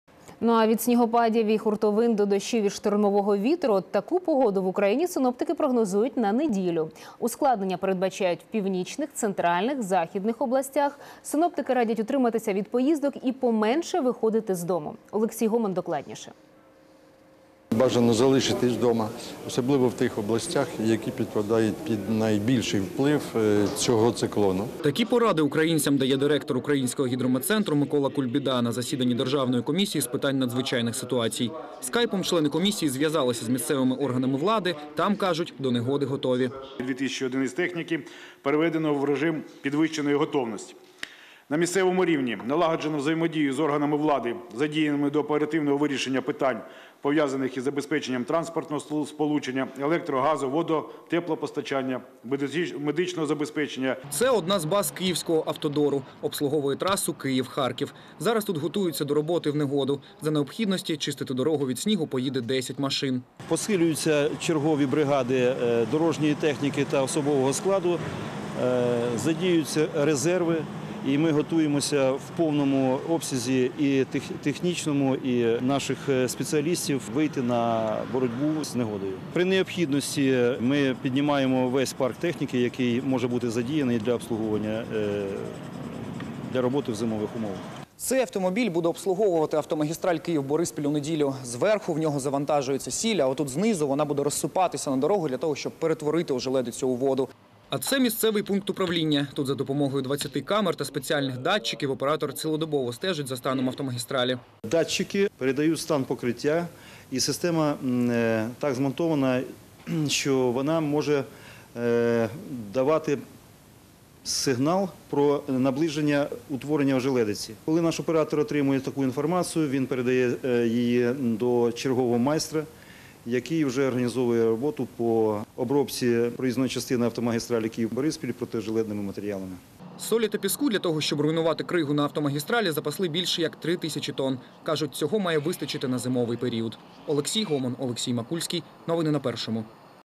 Снігопадіи і хуртовини синоптики прогнозують на неділю
Ускладнення передбачають у північних, центральних і західних областях. Синоптики радять утриматися від поїздок і поменше виходити з дому. Олексій Гомон докладніше.